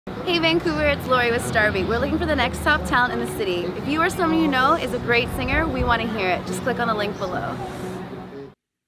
StarBeat™ - Talent Search: Vancouver Singers
We are searching for the Top Talent in Vancouver!

Are you a top Singer? If you or someone you know are a Top Talent in Singing then we want to see it.

Please check out the links below to submit your talent video to our StarBeat™ Social Talent Contest!

The deadline is August 31, 2016 so enter your video now!